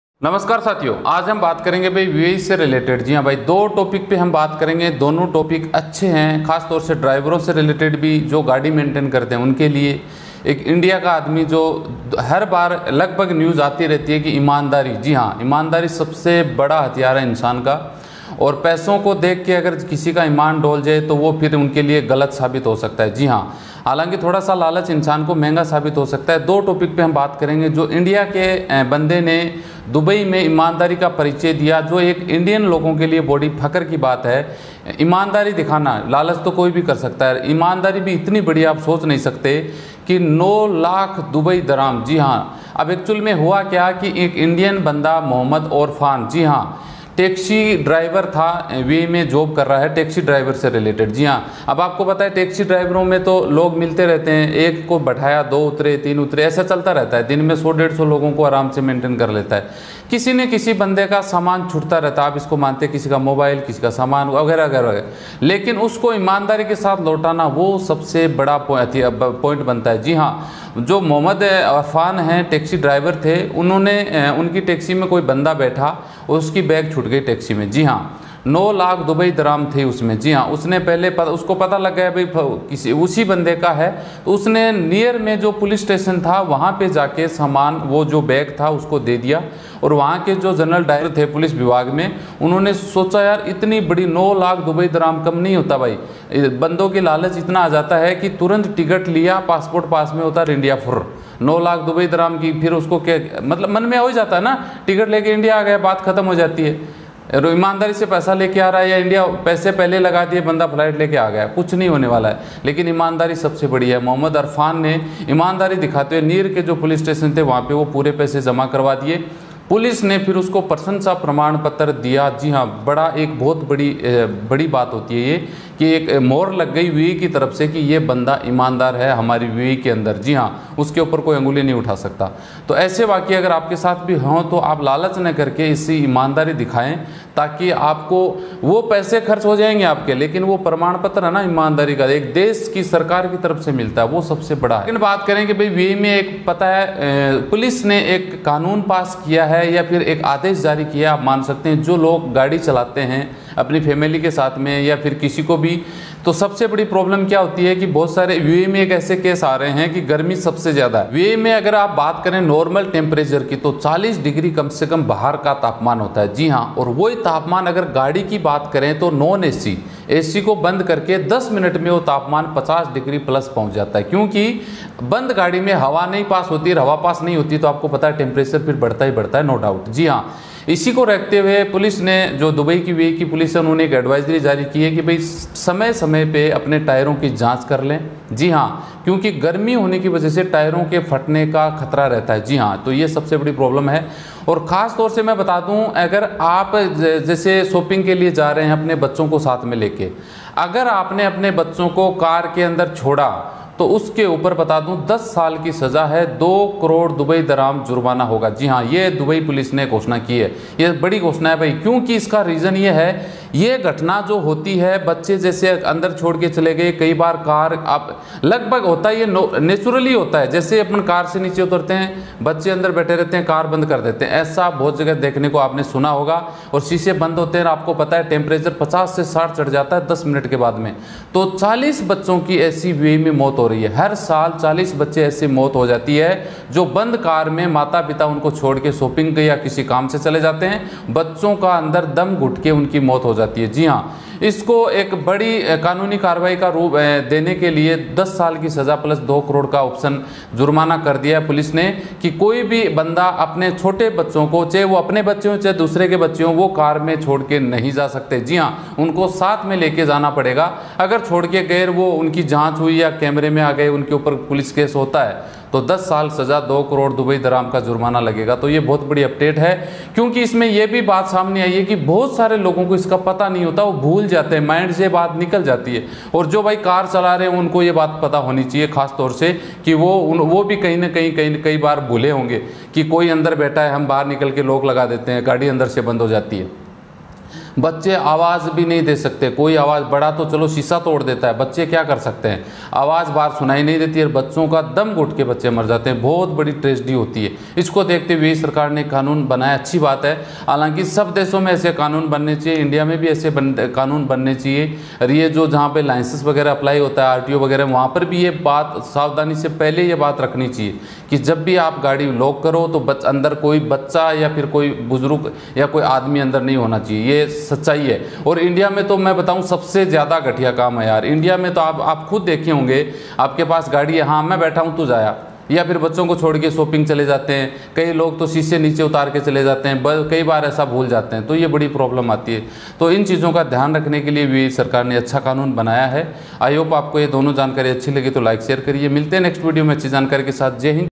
दुबई में इंडियन ड्राईवर मोहम्मद ओसफ ने दिखाई बड़ी ईमानदारी एक करोड़ 80 लाख रुपए।dubai emirates id.
IFSC CODE-SBIN0031123
GOOGLE PAY NO.9414503812
PAYTM NO.9414503812
PHONE PAY NO.9414503812
CALL TIME-10AM-5PM
SUNDAY CLOSED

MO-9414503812

In Dubai, Indian driver Mohammad Osaf showed great honesty and returned Rs 18 million.,